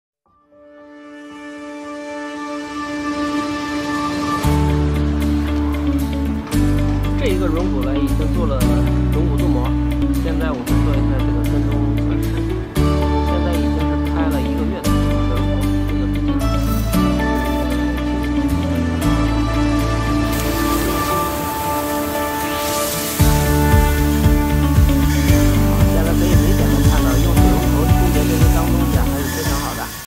Automobile wheel hub coating Effect comparison
Effect comparison Test results for 30 days after driving